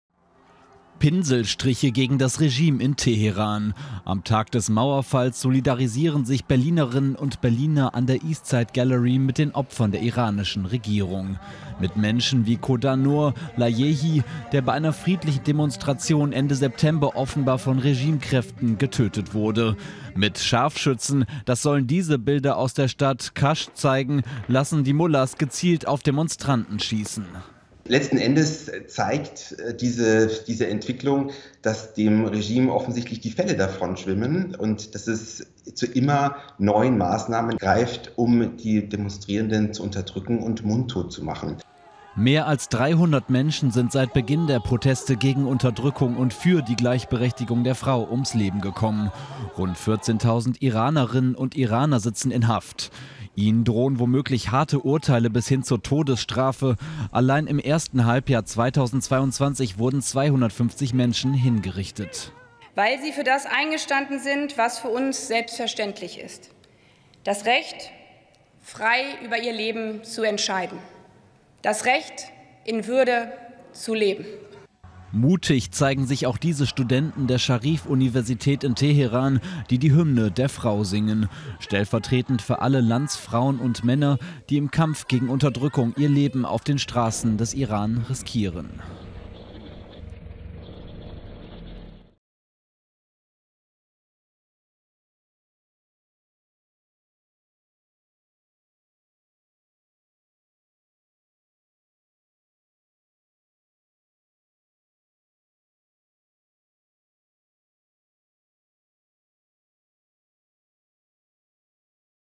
IRAN: Bislang mehr als 300 Tote - Mullah-Regime lässt offenbar gezielt auf Demonstranten schießen
Im Iran sind Sicherheitskräfte Berichten zufolge wieder gewaltsam gegen Proteste vorgegangen. Im Südosten demonstrierten zahlreiche Menschen mehrere Wochen nach der blutigen Niederschlagung von Protesten in der Stadt Sahedan, wie Augenzeugen berichteten. Am 30. September waren in der Hauptstadt der Provinz Sistan-Belutschistan Dutzende Demonstranten getötet worden. Immer wieder strömen Protestteilnehmer nach der traditionellen Trauerzeit von 40 Tagen auf die Straßen, um ihre Wut und Trauer auszudrücken.

Aus Sorge vor Ausschreitungen waren Berichten zufolge viele Geschäfte in Sahedan geschlossen. Polizei und Sicherheitskräfte reagierten laut Augenzeugen mit Schüssen und Tränengas auf Kundgebungen rund um den Stadtfriedhof. «Das ist nun Alltag», beschrieb ein Mann die Lage. Schon lange befindet sich die Stadt nach dem 30. September, der fortan als «blutiger Freitag» bezeichnet wurde, im Ausnahmezustand. Bewohner sprachen von bürgerkriegsähnlichen Zuständen. Auch in anderen Landesteilen gab es am Mittwoch aus Solidarität Proteste.

Auch ein einflussreicher sunnitischer Geistlicher der Provinz, Maulawi Abdulhamid, kritisierte jüngst den Kurs der politischen Führung in dem mehrheitlich schiitischen Land. Weitere örtliche Geistliche sollen sich seinem Kurs angeschlossen haben. Der Prediger forderte jüngst gar ein Referendum über die Forderung der Demonstranten. Irans Führung in Teheran betrachtet dies mit Sorge.

Auslöser der Proteste im Iran war der Tod der 22 Jahre alten iranischen Kurdin Mahsa Amini Mitte September. Die Sittenpolizei hatte sie festgenommen, weil sie gegen die islamischen Kleidungsvorschriften verstoßen haben soll. Die Frau starb am 16. September in Polizeigewahrsam. Seit ihrem Tod demonstrieren landesweit Zehntausende gegen den repressiven Kurs der Regierung sowie das islamische Herrschaftssystem.